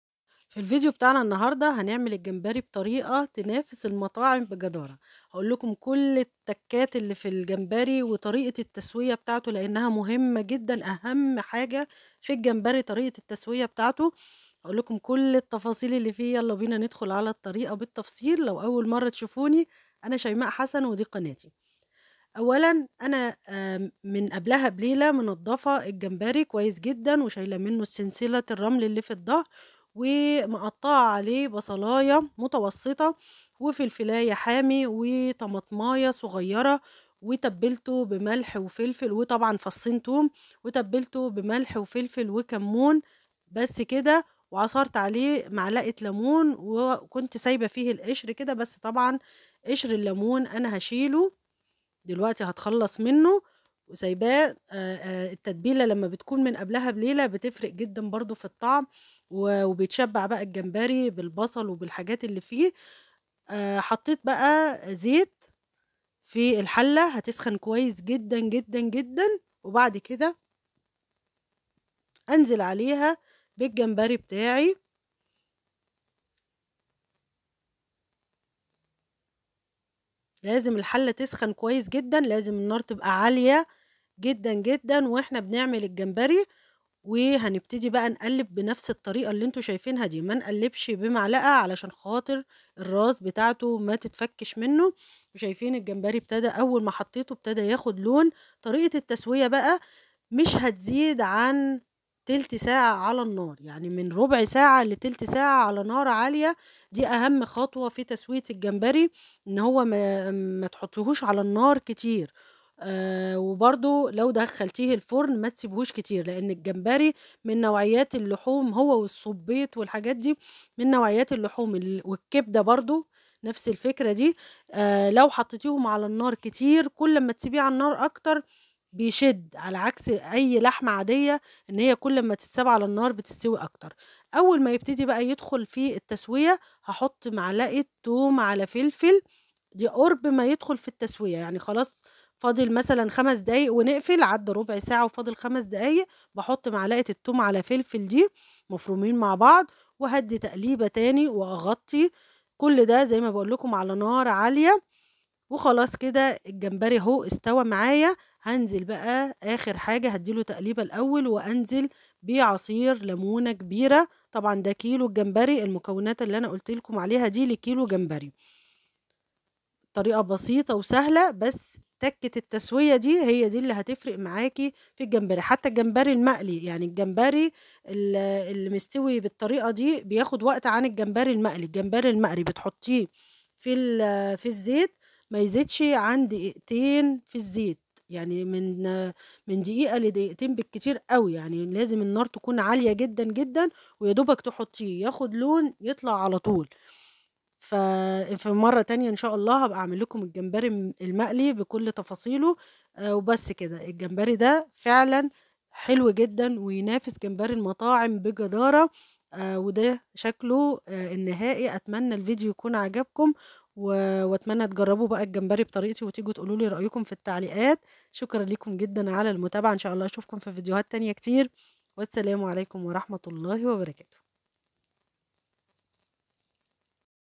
أسهل طريقة لعمل الجمبري
طريقة عمل الجمبري بأبسط طريقة شرح كل التكات البسيطة اللي تخليه يتحدى جمبري المطاعم